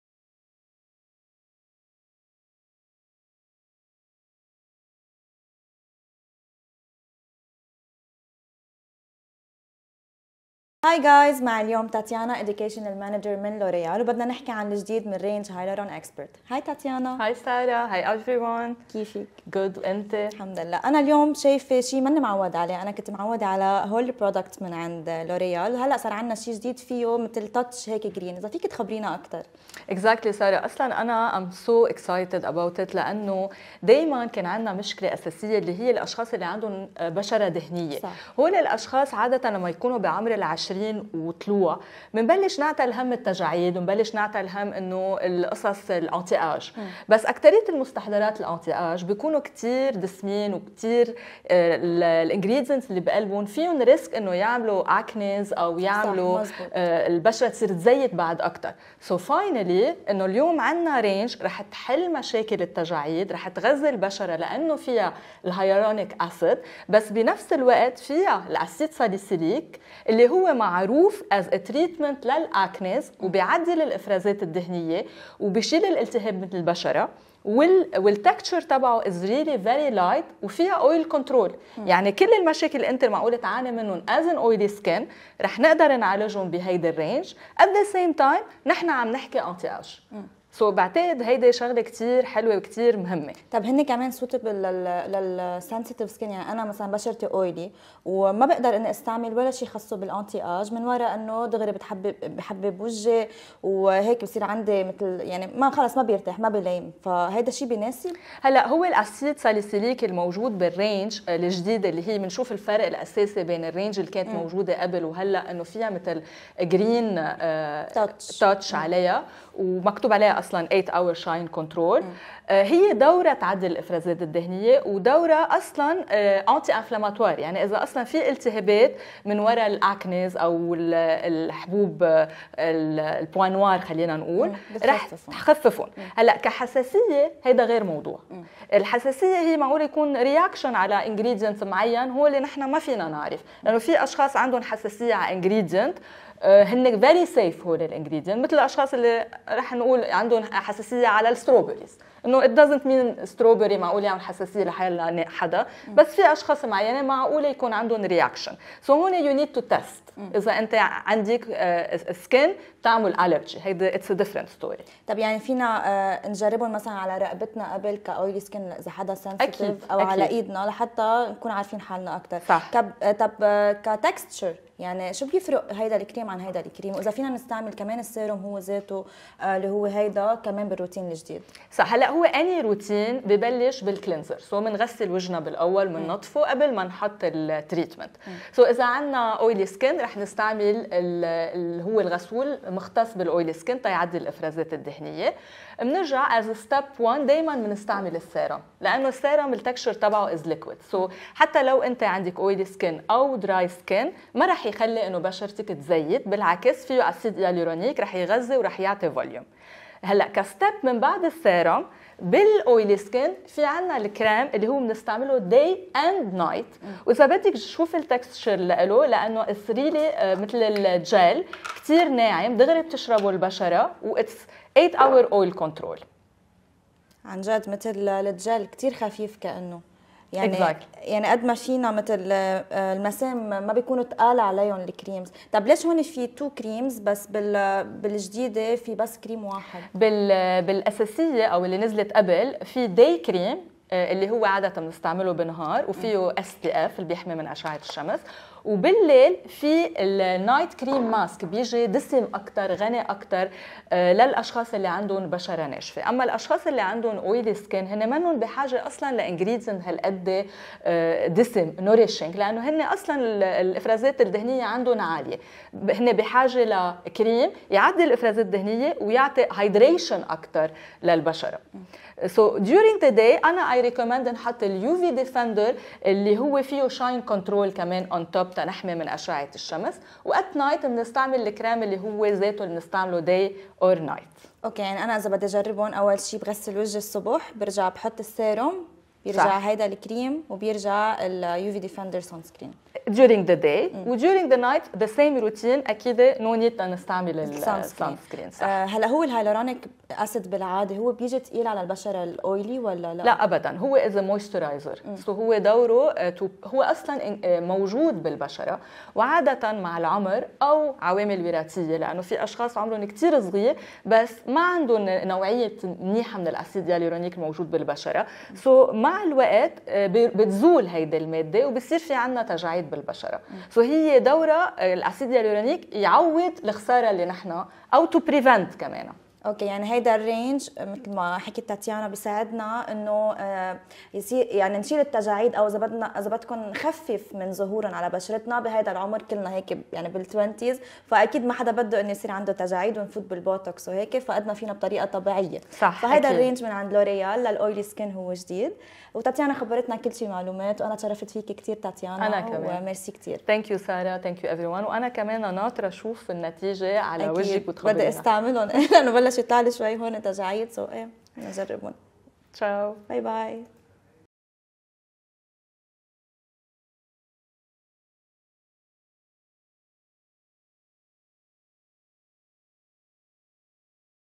The All New Hyaluron Expert OIL CONTROL
Discover the all new Hyaluron Expert Oil Control products for Combination to Oily Skin with Tatiana and Sara!